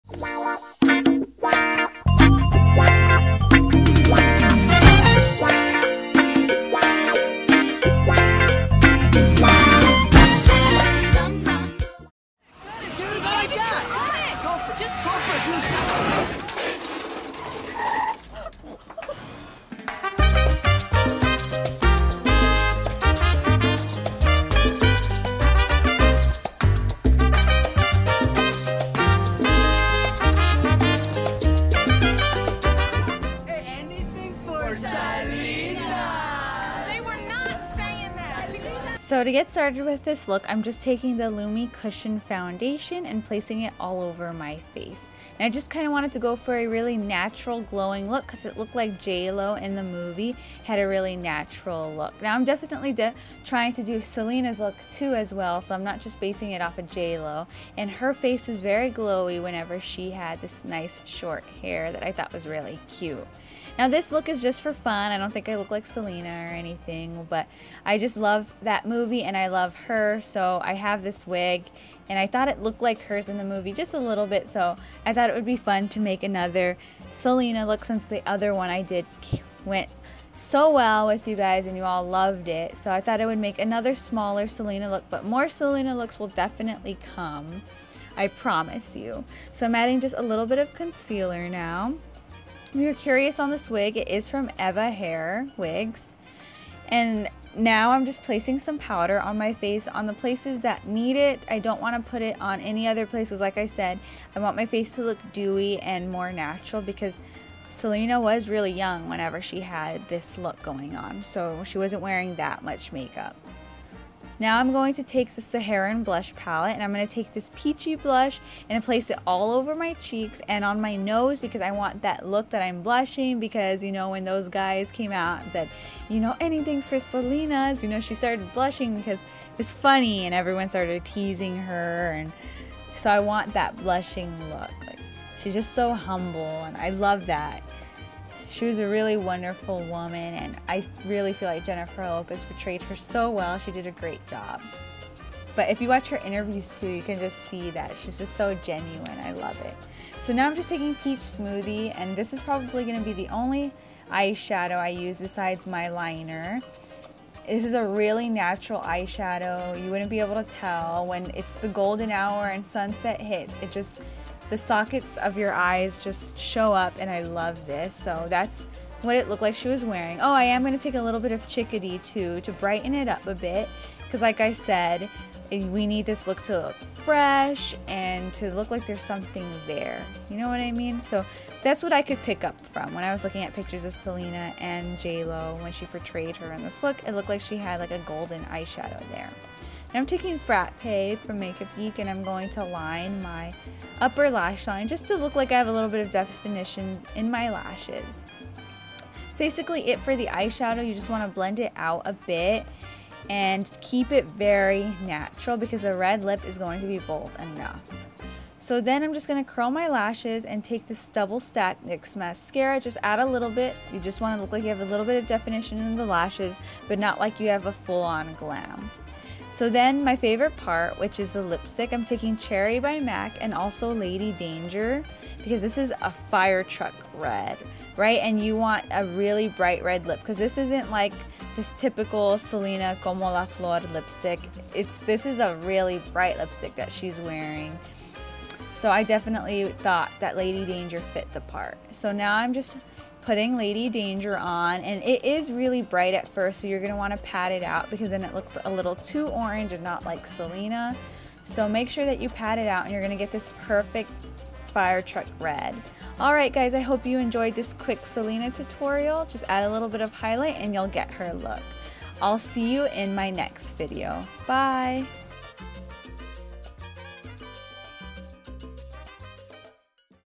Anyyythingg for Selenasss Makeup Tutorial 🌹 | Selena Q
Hello my loves! I thought that it would be fun to do another Selena Quintanilla inspired look but this time a little more young & fresh. Selena was always known for her bold red lip so I decided to go for that again, heavily inspired by the look Jennifer Lopez wore while she was playing Selena. I hope you all enjoy this cute quick video!!